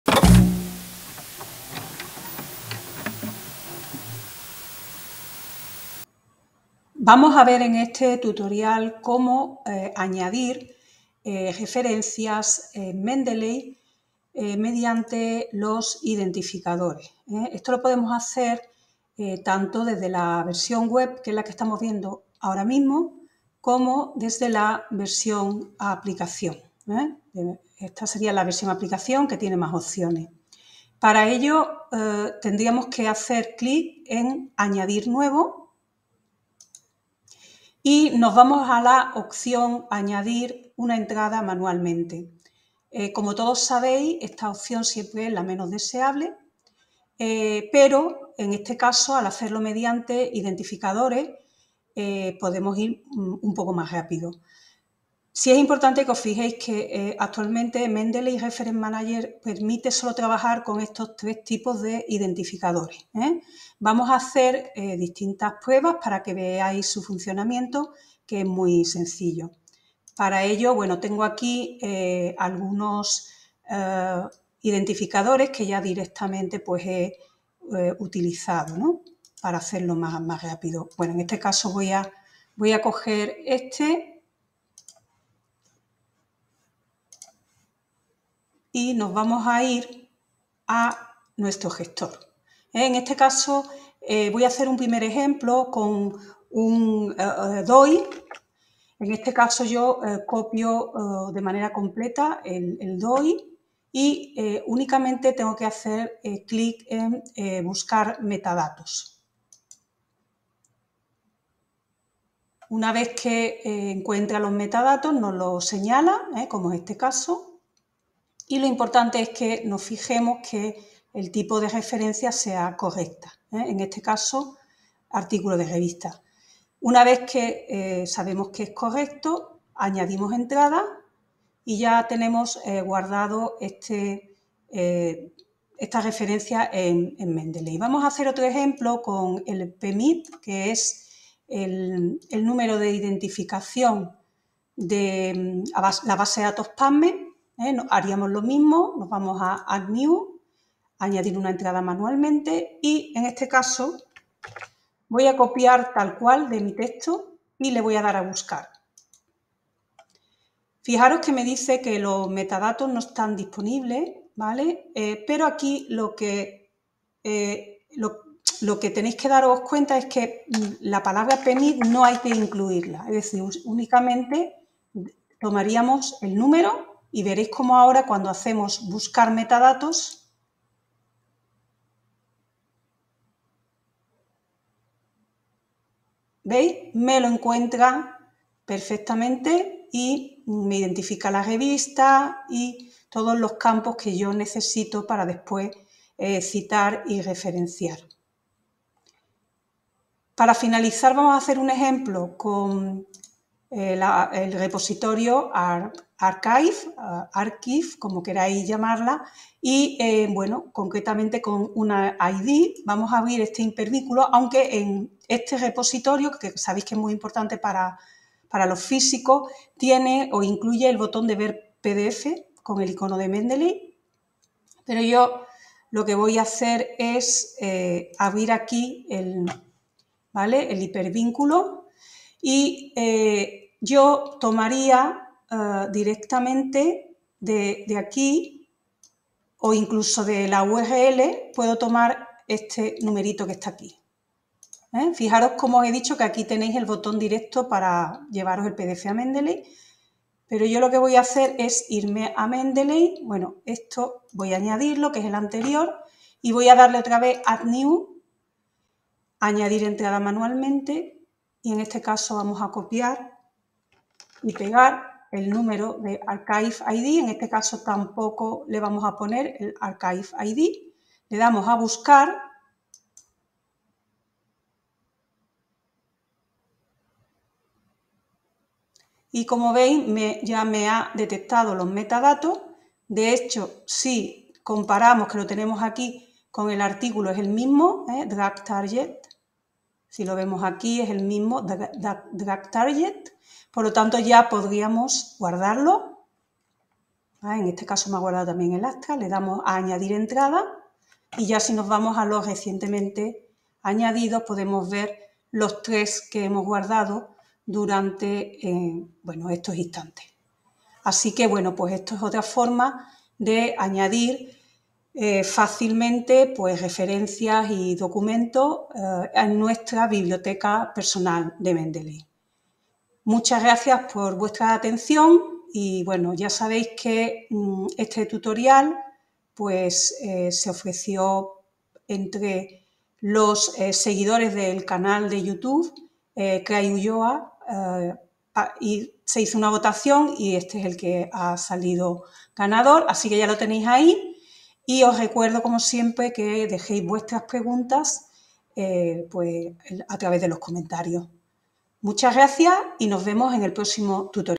Mendeley Reference Manager : añadir referencias mediante identificadores
Los gestores de referencias permiten añadir las referencias y documentos de manera manual. MRM nos permite además dentro de esta opción trabajar mediante identificadores como el DOI, PMID o ArXiv ID. Con este vídeo podréis aprender estos procedimientos.
Realizado con Clipchamp.